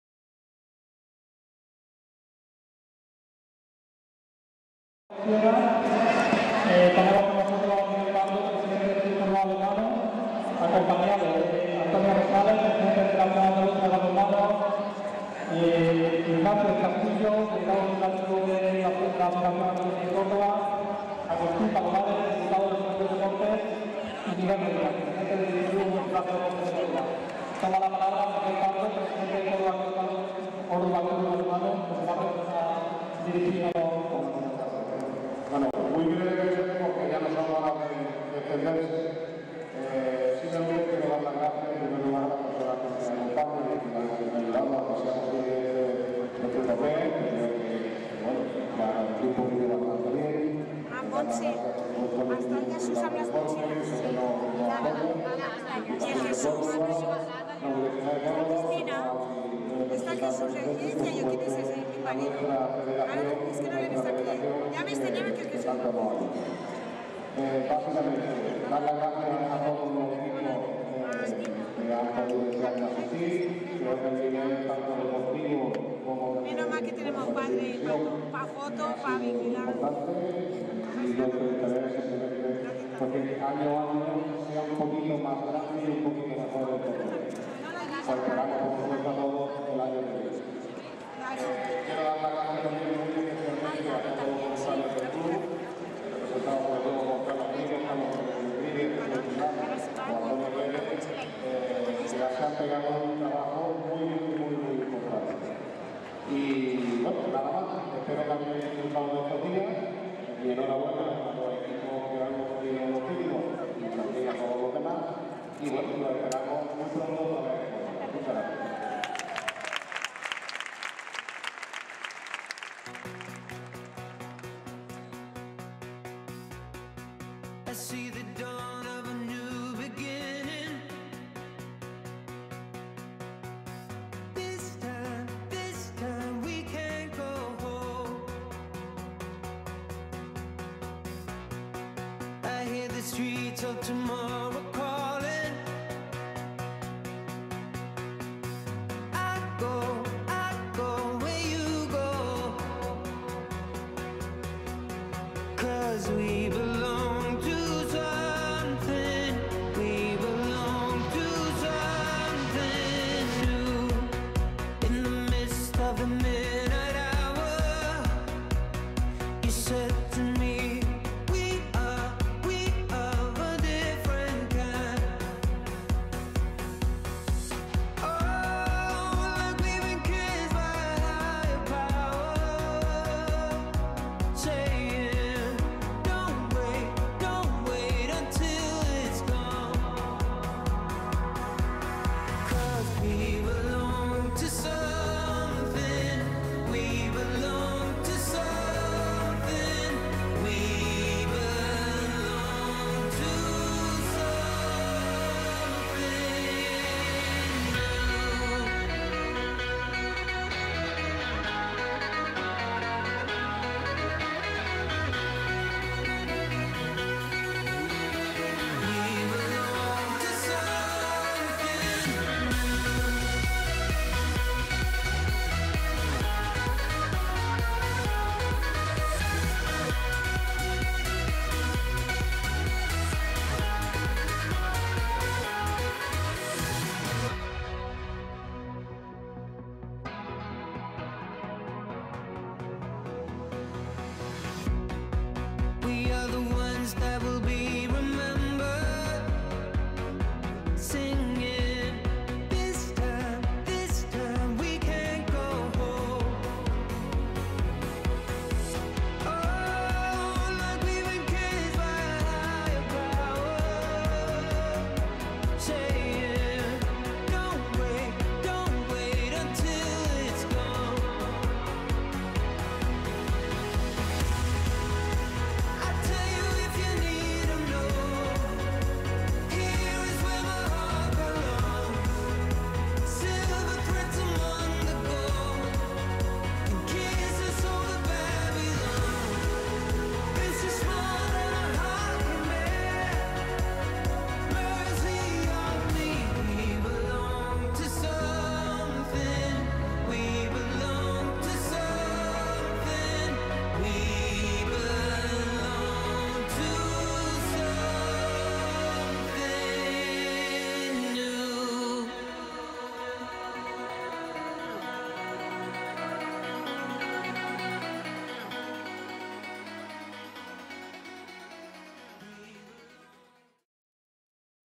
ENTREGA TROFEOS CÓRDOBA HANDBALL CUP 2015
Ceremonia de clausura y entrega de trofeos a los vencedores de todas las categorías en la VII edición de la Córdoba Handball Cup. Acto celebrado en el Palacio Municipal de Deportes Vista Alegre el 1 de abril de 2015.